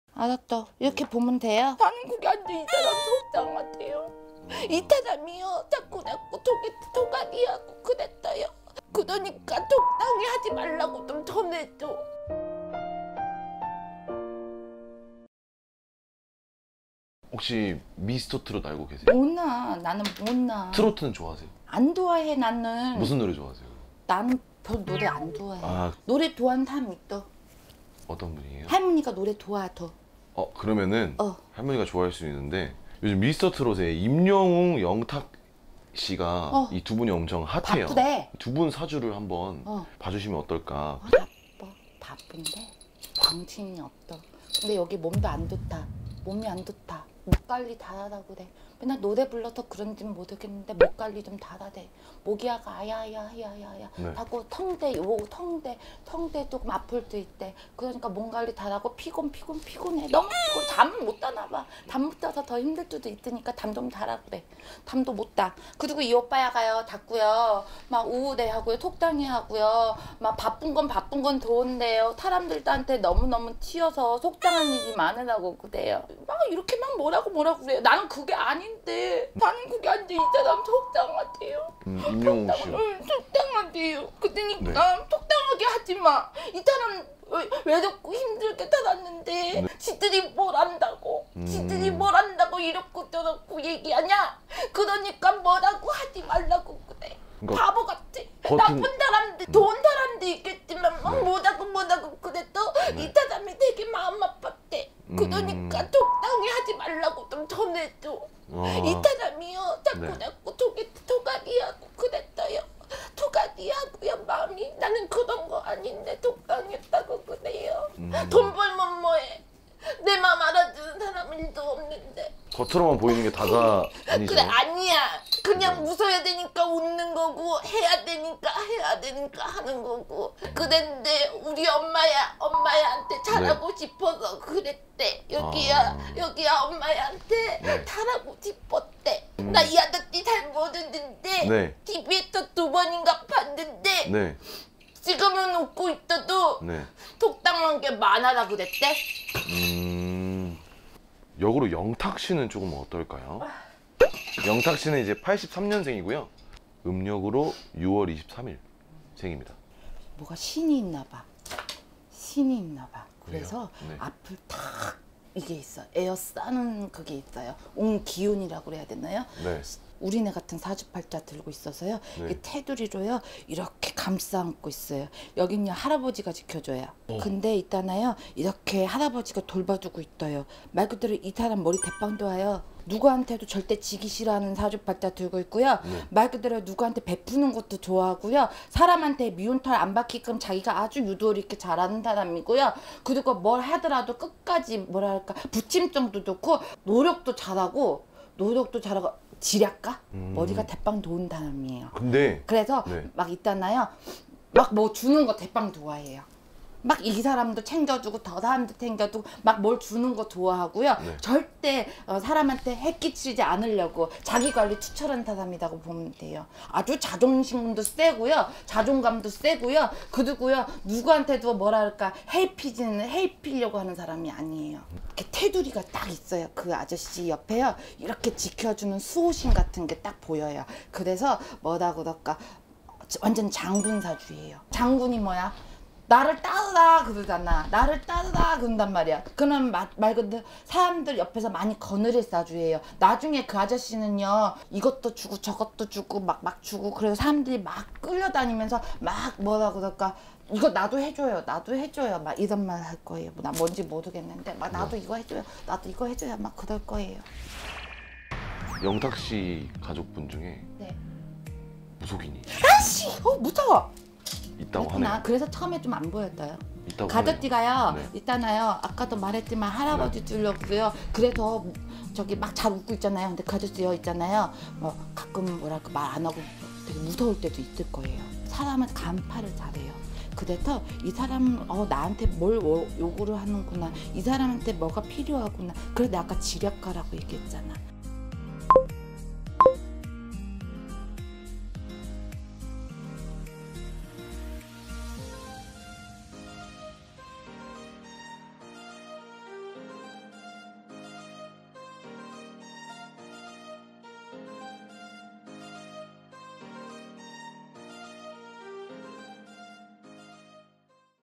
애동제자가 보는 '미스터트롯' 임영웅 vs 영탁 사주배틀 촬영중에 눈물까지 흘리다.... 서울점집 선녀대신
자막

★대표블로그★
-올마이티TV